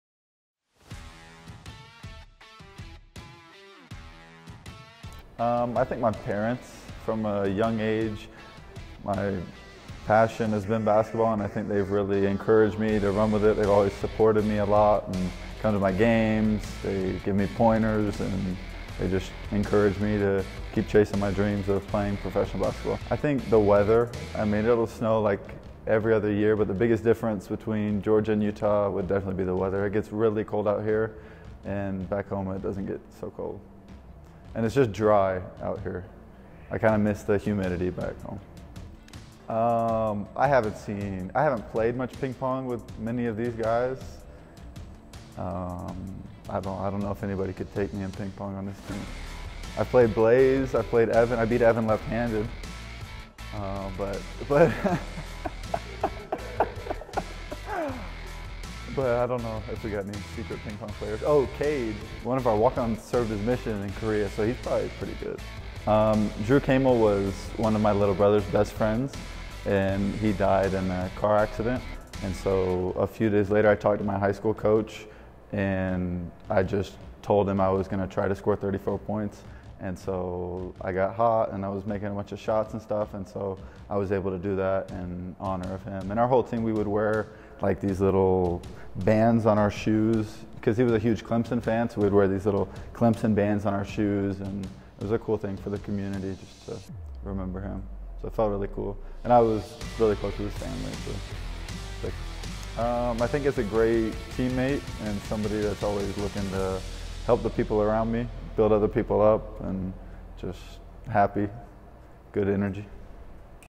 UVU Basketball: Colby Leifson Profile
This Georgia native and BYU transfer is second to none on the team in Ping Pong.